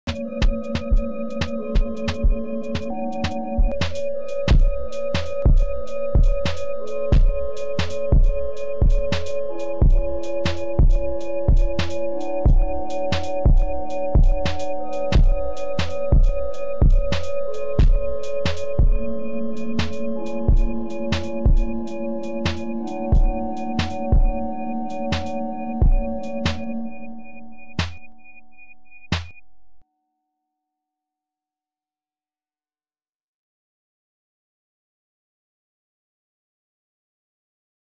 DONT WATCH VERY BADDD
The music is not mine the video is mine I was going to make a mini movie but what I made sucked Xd